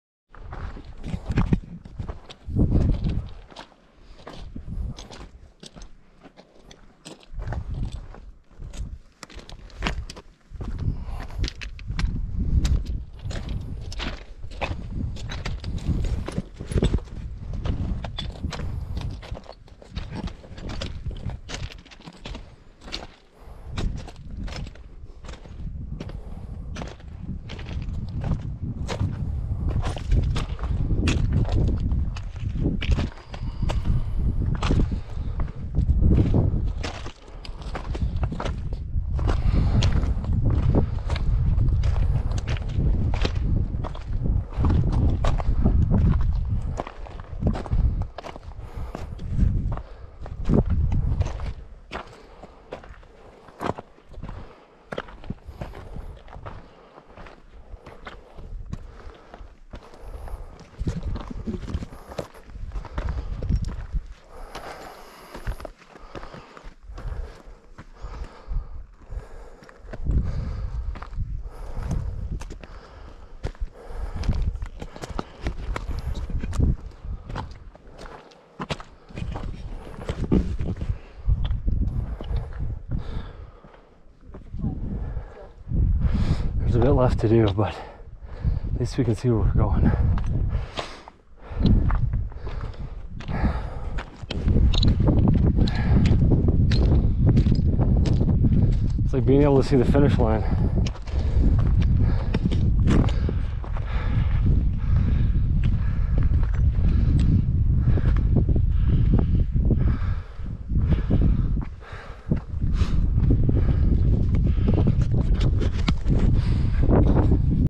Mt Burke mountain hike ascent 2
Ascending to the top over narrow rocky ridge